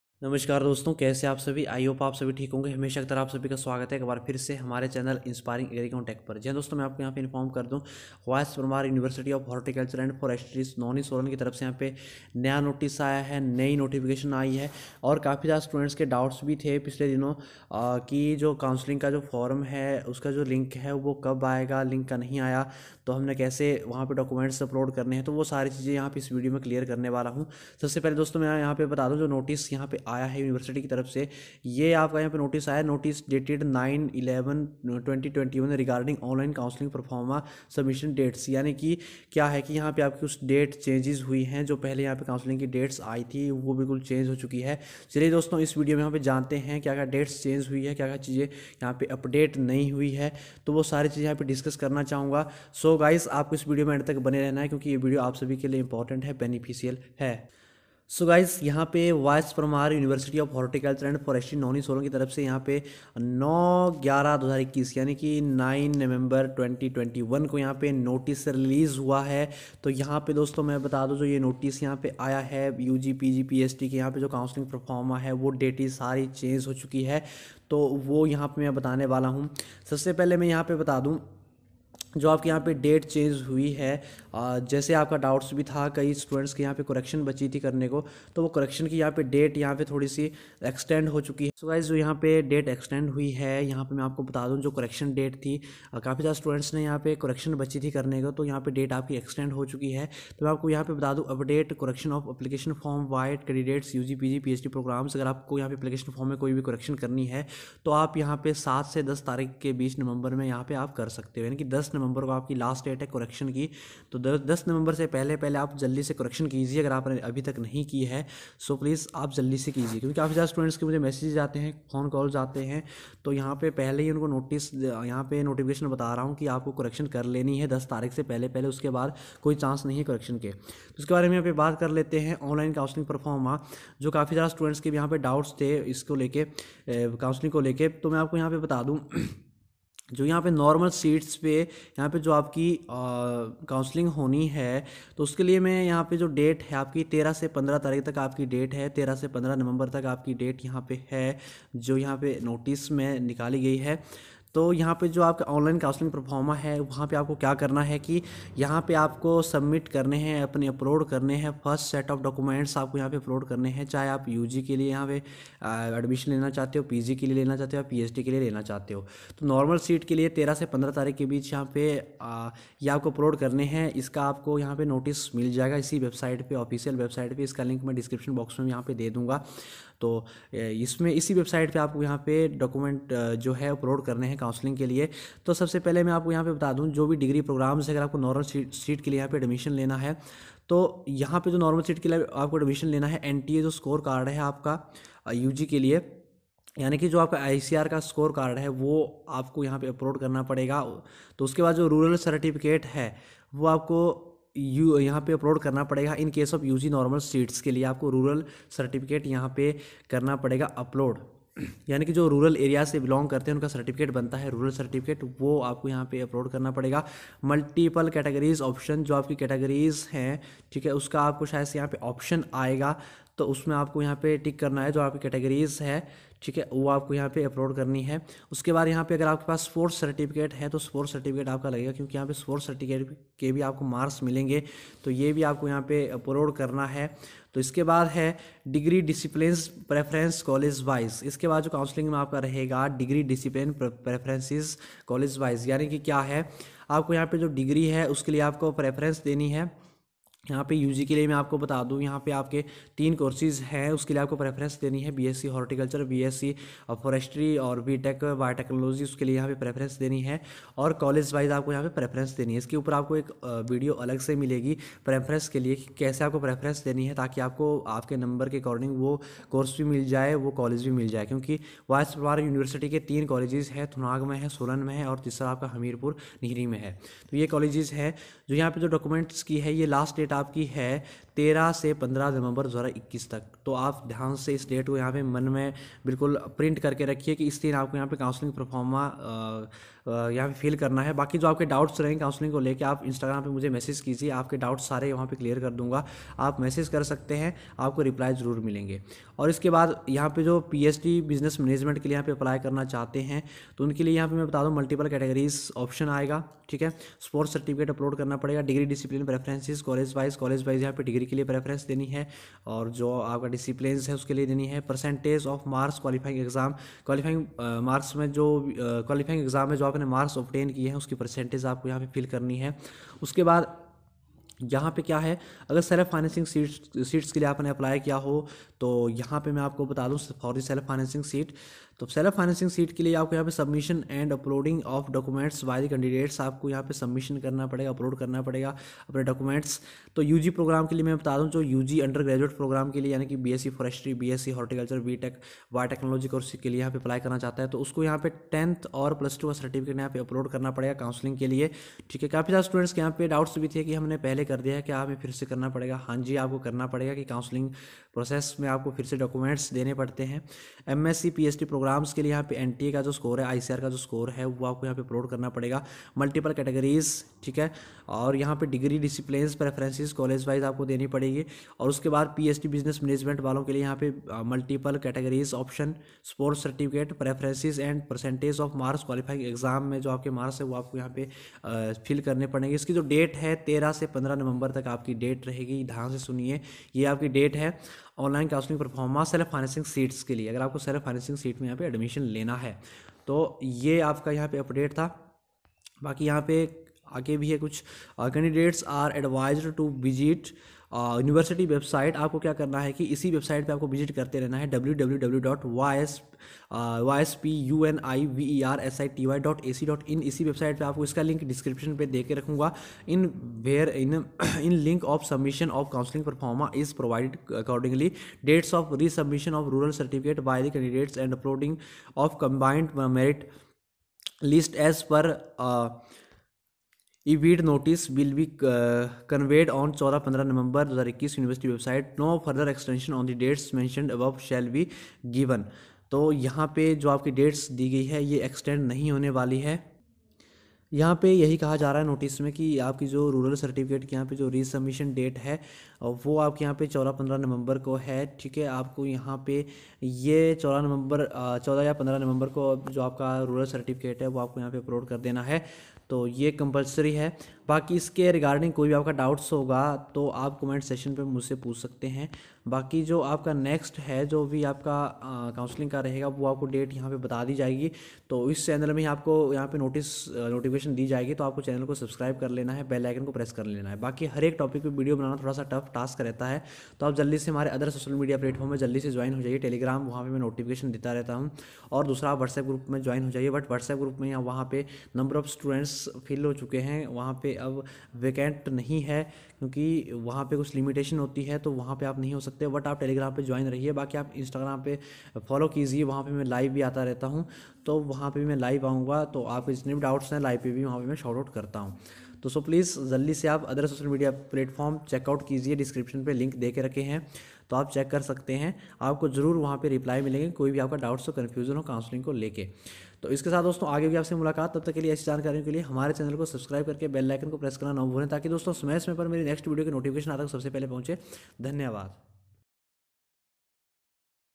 Ys parmar counselling new update 2021 / Ys parmar Official notice / counselling date extended
Ys parmar university ug/pg/ PhD counselling date schedule released | Official notice for counseling

Follow me other social media platforms:-